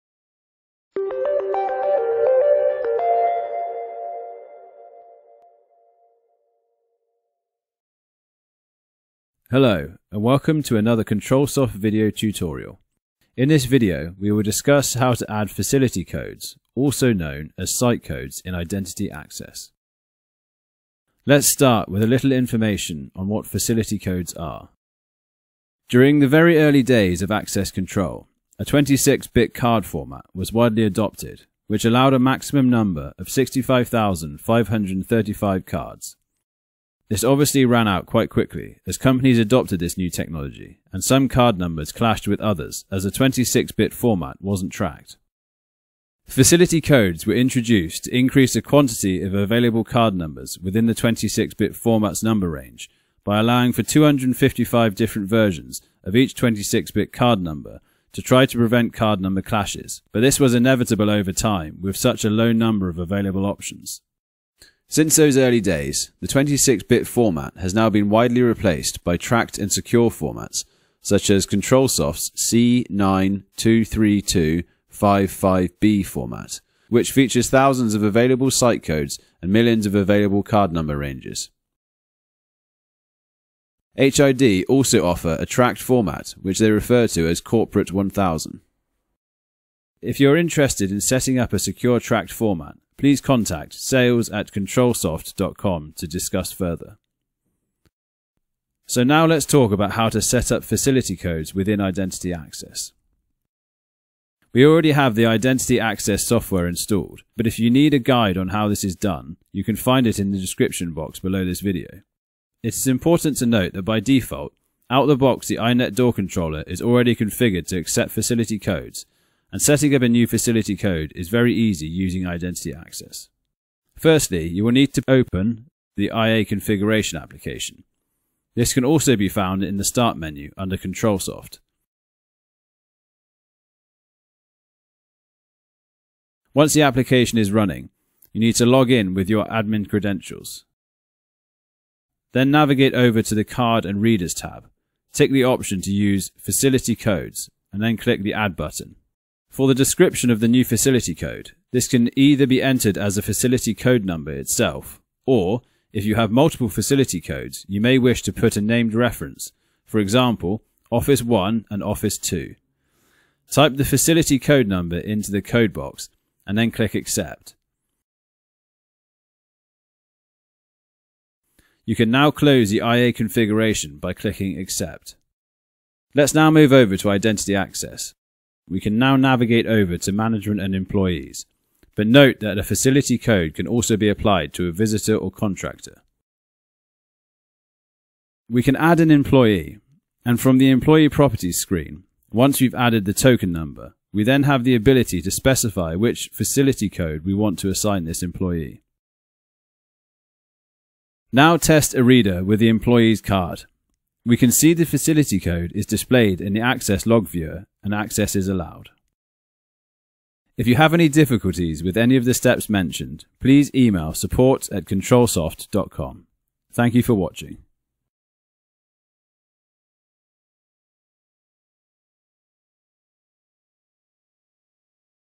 How to add Facility Codes in Identity Access (Controlsoft)
In this video, we explain how to add and configure Facility Codes in Identity Access access control system.

If you are interested in the Identity Access platform or have any questions, please don’t hesitate to get in touch:

EMEA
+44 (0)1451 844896

papillon

South Africa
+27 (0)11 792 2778